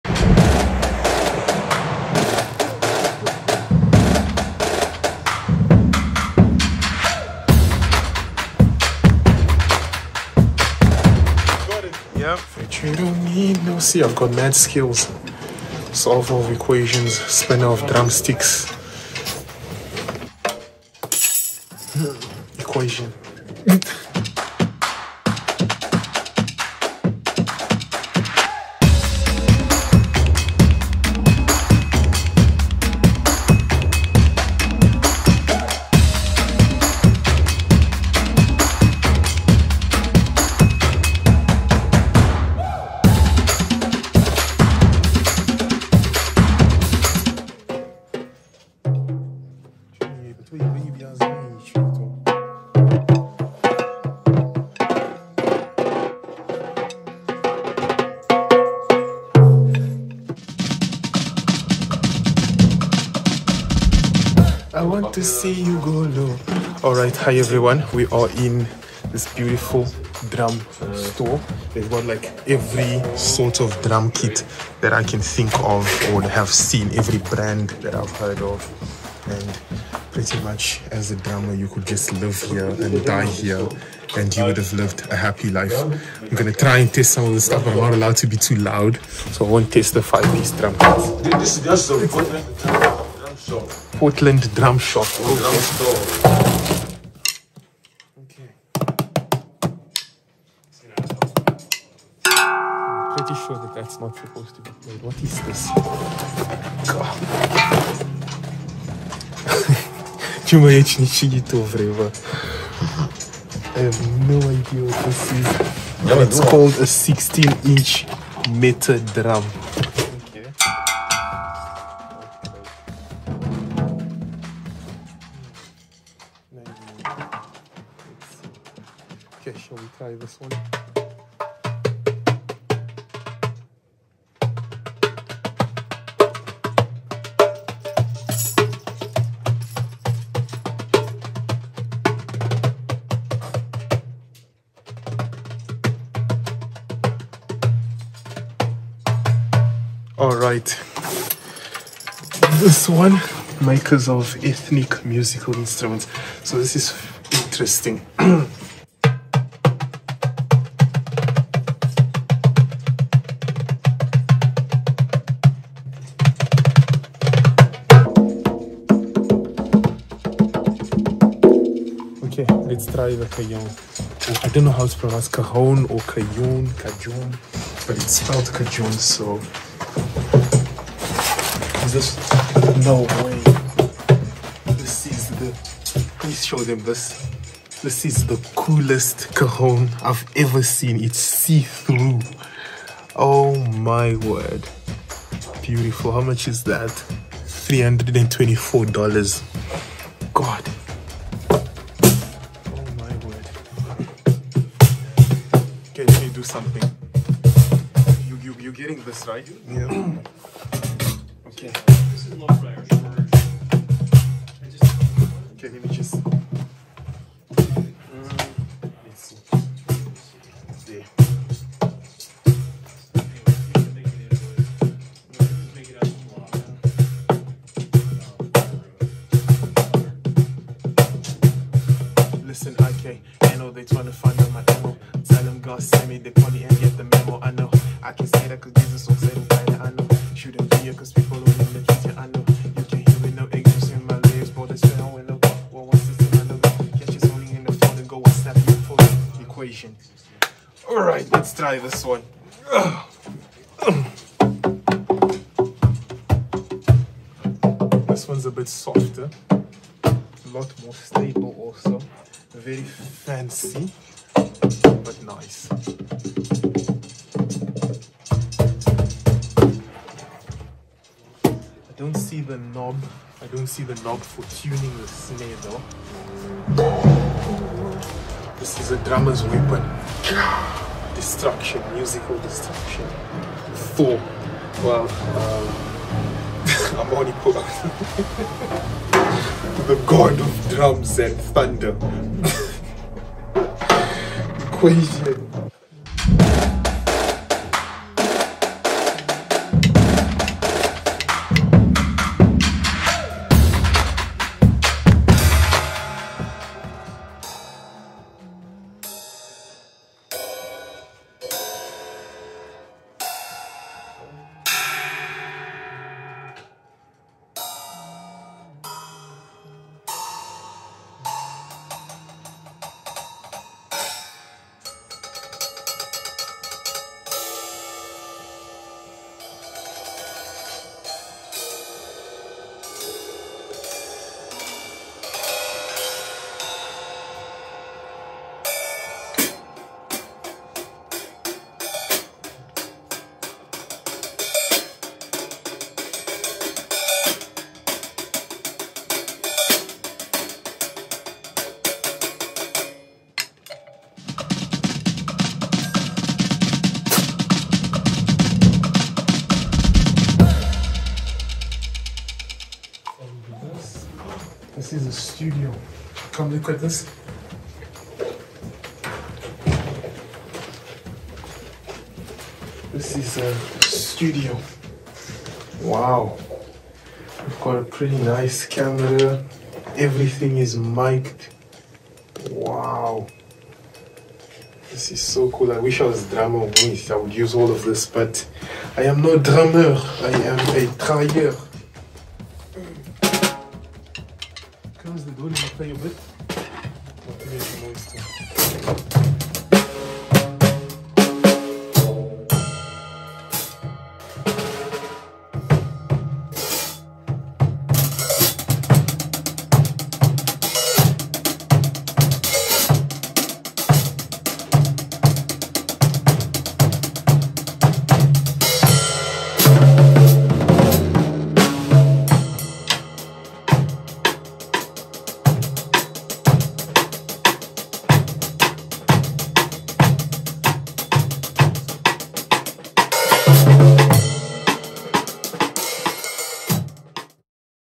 Drama in a Drum Shop | Portland Maine, USA
Hi there Equationnaires!

To most of us a drum is a drum really. Yet, it surprises even me who is a musician of sorts, to see that there is an entire store that is just about drums. There are sooooooooooo many types of drums, so many forms, shapes and sizes. Some of them I didn't even recognize as drums! Hence the Drama, I mean Drummer!

I actually first start learning how to play the drums, so drums were my intro into music, then I moved to Bass guitar and then guitar then the microphone!

Stay Blessed!